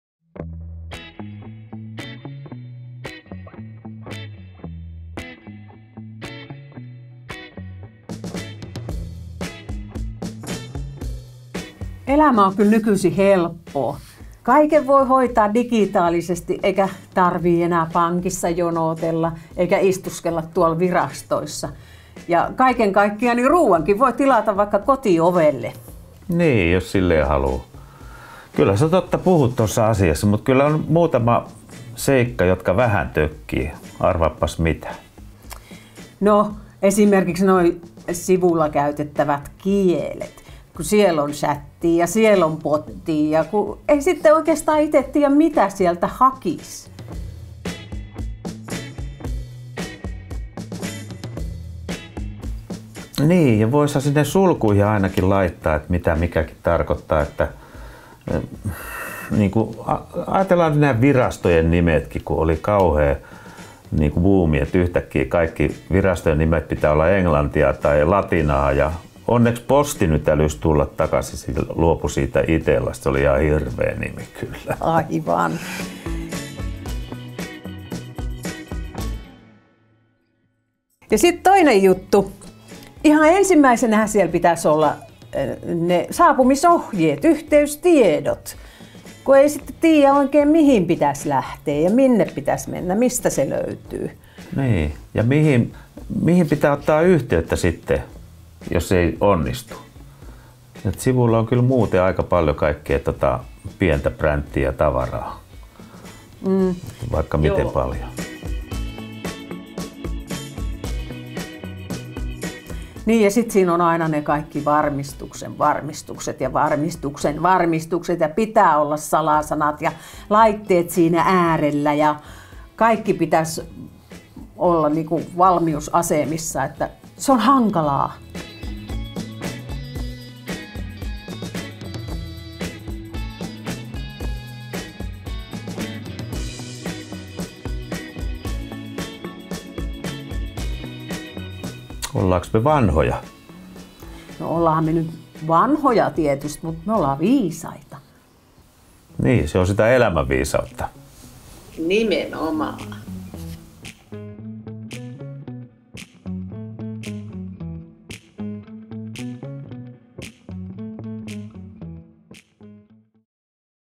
Sujuvampaa digielämää
On chattia ja bottia. Mitä nämä kaikki oikein ovat? Digitaalisuudesta on iloa monille ja se helpottaa useiden elämää. Mutta vaikka digipalveluita käyttäisi säännöllisesti, ei digimaailman kieli ole tuttu kaikille. Voisikohan sinunkin organisaatiosi saada palveluista entistä selkeämpiä sekä käytettävyydeltään että kieleltään?

Video on Vanhustyön keskusliiton SeniorSurf-toiminnan tuottama. SeniorSurf tukee ikäihmisten digiosallisuutta ja digitaitojen kehittämistä.